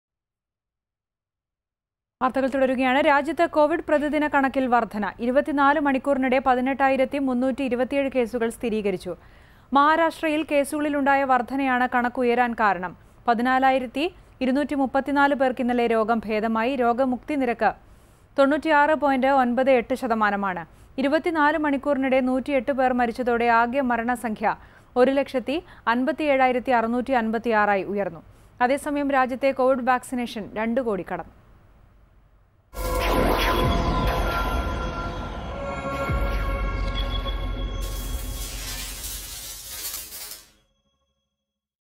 കോവിഡ് വ്യാപനം ഉയരുന്നു; രോഗവ്യാപനം കൂടുതൽ മഹാരാഷ്ട്രയിൽ | Covid19
Covid spread is increasing. Maharashtra has highest spreading rate.